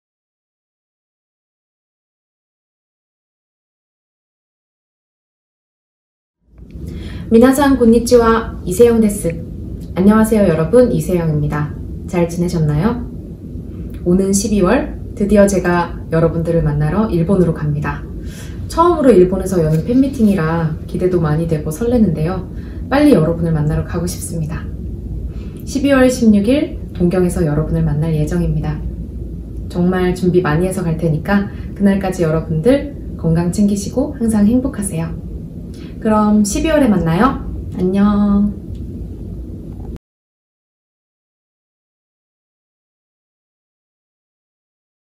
【イ・セヨン】ファンミーティングに向けコメント動画が到着！
日本で初のファンミーティング開催！「2023 イ・セヨン ファンミーティング」

12/16(土) ヒューリックホール東京